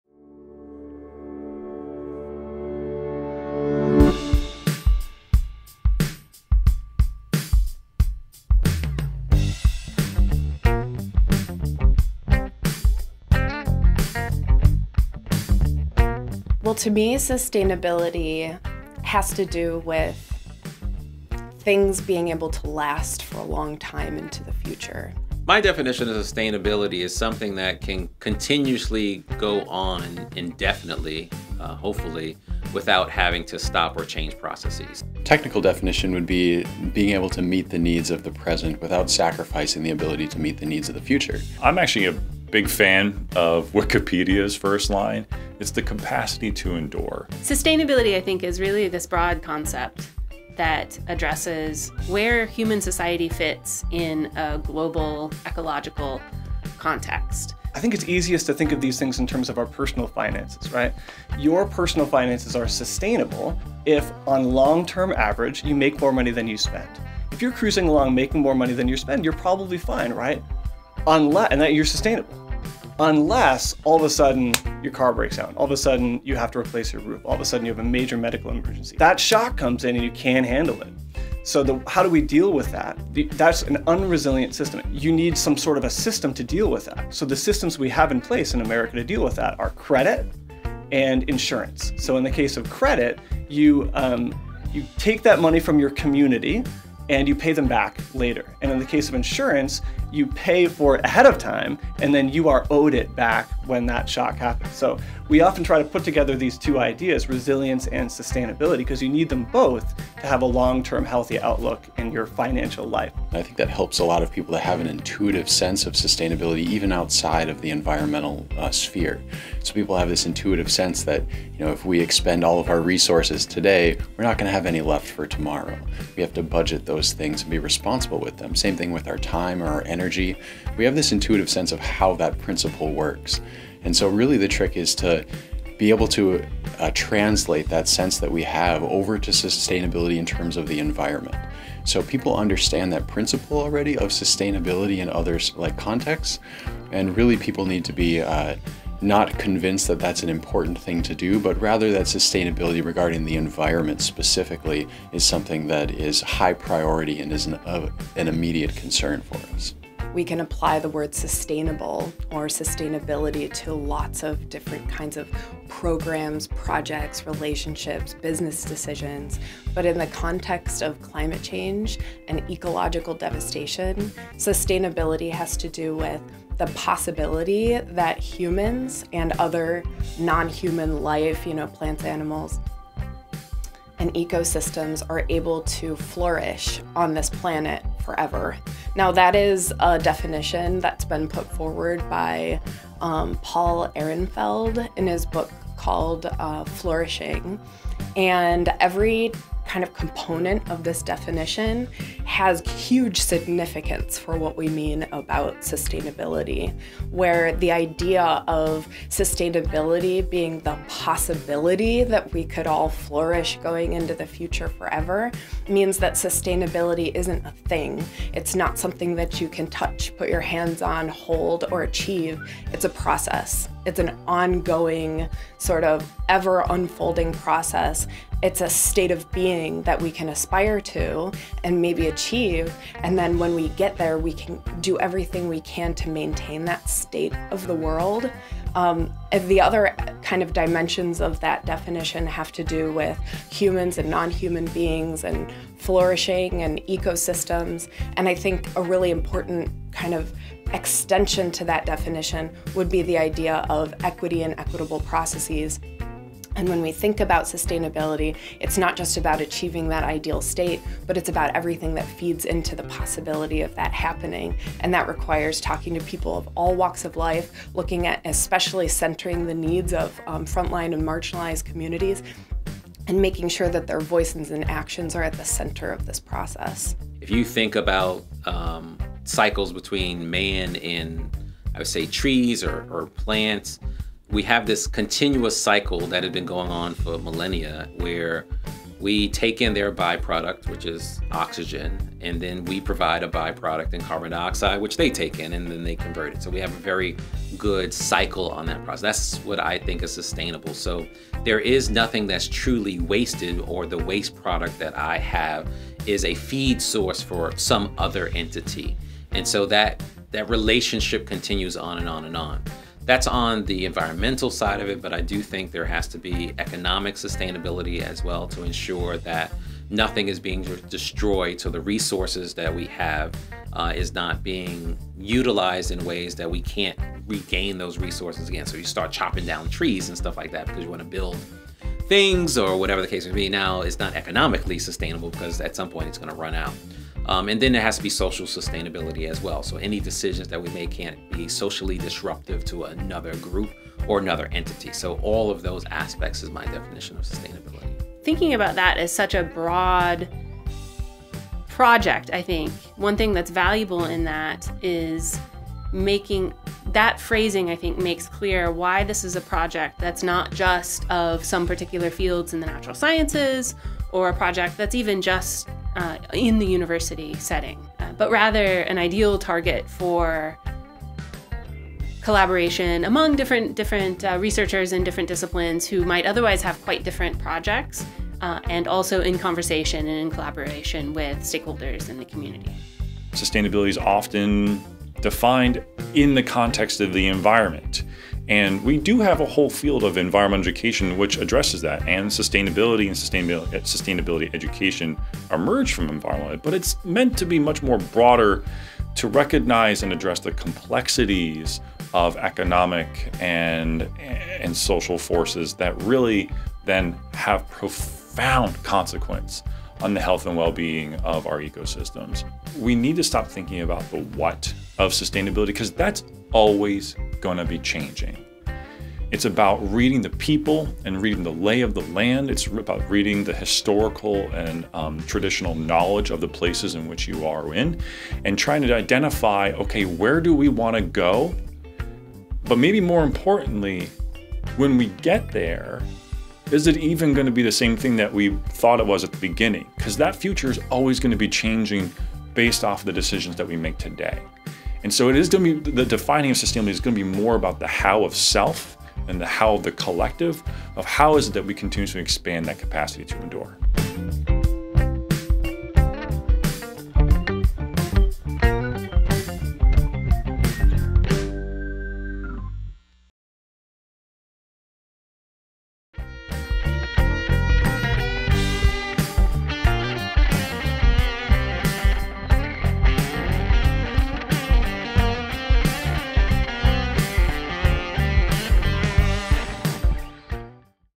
What is Your Definition of Sustainability? | Sustainability Highlights Interview Series
Dr. Carlie Trott (UC Psychology), Dr. James Mack (UC Chemistry), Dr. Abel Gustafson (UC Communication), Dr. Andrew Bernier (UC Curriculum & Instruction), Dr. Angela Potochnik (UC Philosophy), and Dr. Patrick Ray (UC Environmental Engineering & Science) offer their thoughts on the definition of sustainability.

Videos recorded February--March, 2022. All applicable state and institutional masking and vaccine requirements were followed in the production of this video. Produced and edited by Lucas Dunlap and Ezekiel King Phillips.